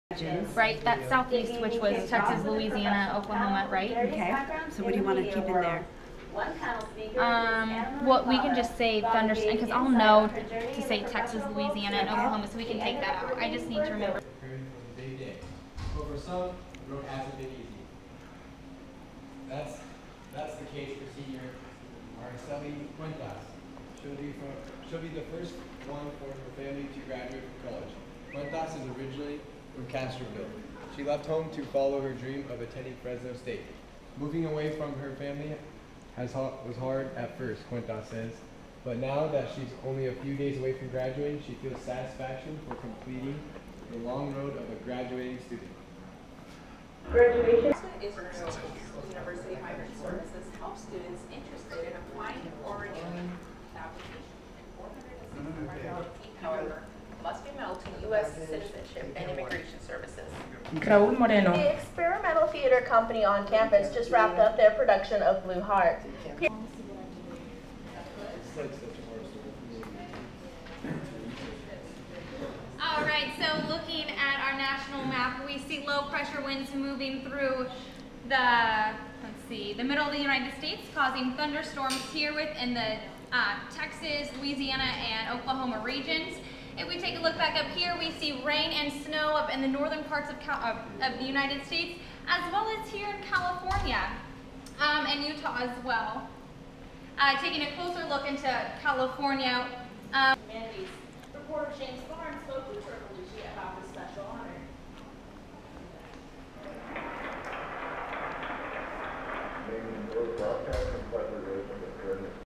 Fresno State Focus "Behind The Scenes"
A behind the scenes look at the production of Fresno State Focus, a
weekly 30 minute newscast. Produced by Fresno State Mass Communications and Journalism students in Studio A.